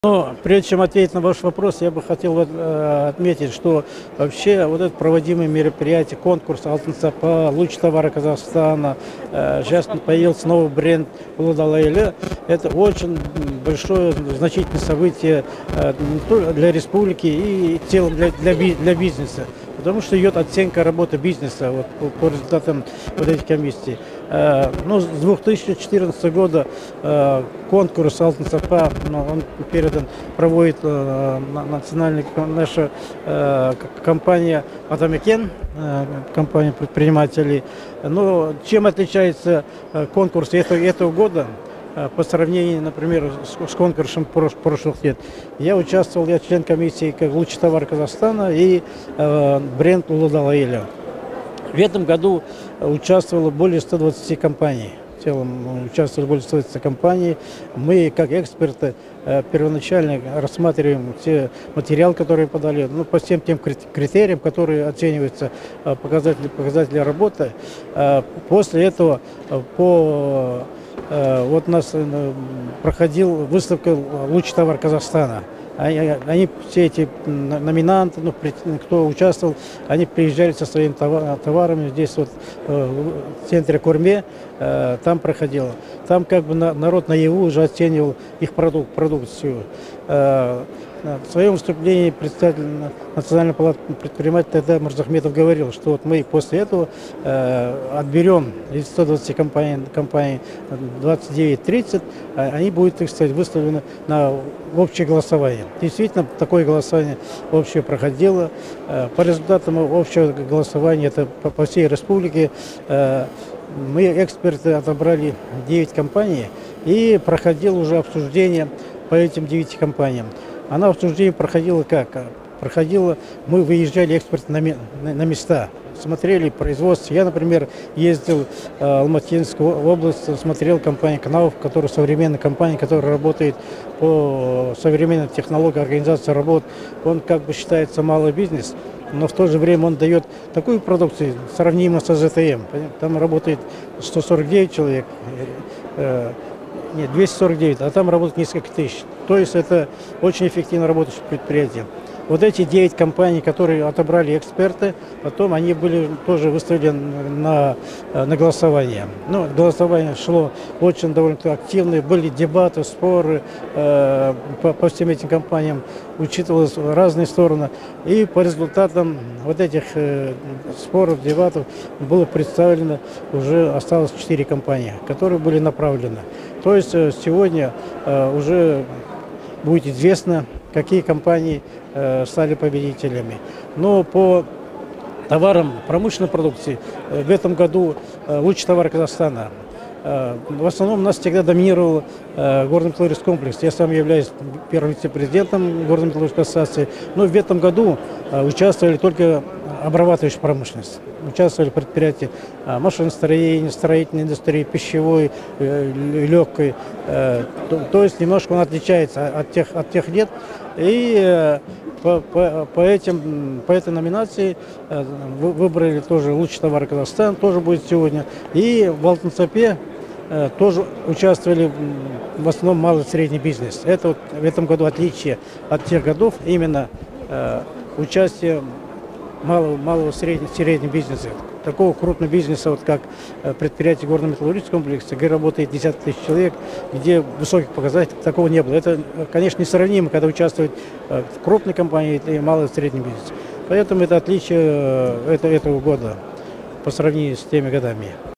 Алтын Сапа 2018.
"Алтын Сапа" присуждается ежегодно на конкурсной основе за достижение организацией значительных результатов в области качества продукции и/или услуг, обеспечения их безопасности, а также за внедрение организацией высокоэффективных методов управления качеством.

Вручение премии Президента Республики Казахстан за достижения в области качества является ключевым событием года. Премию вручает Президент Республики Казахстан Нурсултан Назарбаев, что говорит о большой значимости этого конкурса для страны.